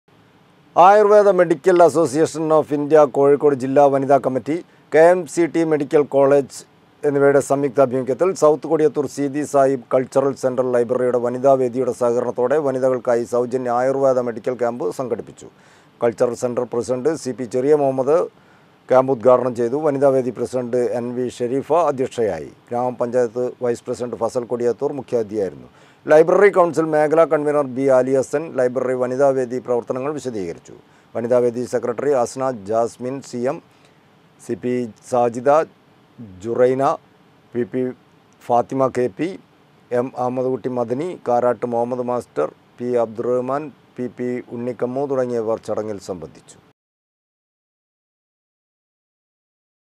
ലൈബ്രറിയുടെ വനിതാ വേദിയുടെ സഹകരണത്തോടെ വനിതകൾക്കായി സൗജന്യ ആയുർവേദ മെഡിക്കൽ ക്യാമ്പ് നടത്തി.
ആയുർവേദ മെഡിക്കൽ അസോസിയേഷൻ ഓഫ് ഇന്ത്യ,കോഴിക്കോട് ജില്ലാ വനിതാ കമ്മിറ്റിയുടെ നേതൃത്വത്തിൽ KMCT മെഡിക്കൽ കോളേജും ചേർന്ന് സൗത്ത് കൊടിയത്തൂർ സീതി സാഹിബ് കൾച്ചറൽ സെന്റർ ലൈബ്രറിയുടെ വനിതാ വേദിയുടെ സഹകരണത്തോടെ വനിതകൾക്കായി സൗജന്യ ആയുർവേദ മെഡിക്കൽ ക്യാമ്പ് നടത്തി.